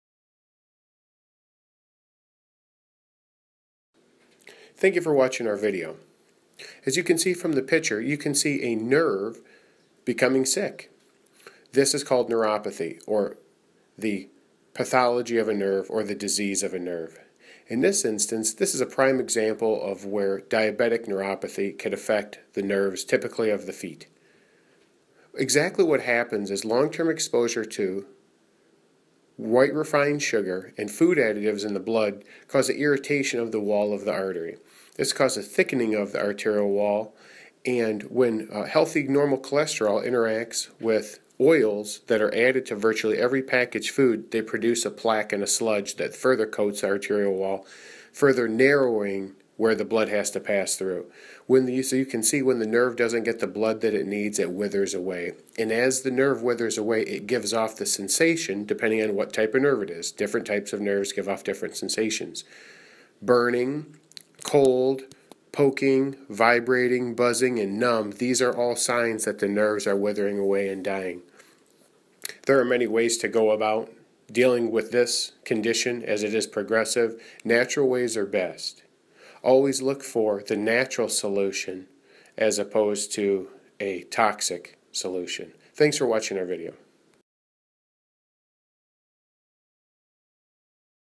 What Causes Nerve Pain, Neuropathy & Fibromyalgia?
By: Dr. Daniel Farkas DC, CFMP.

This video describes what causes neuropathy, the effects on the body and all forms of support available, paying special consideration to holistic and natural support, diet changes, and natural supplementation that may greatly improve the condition.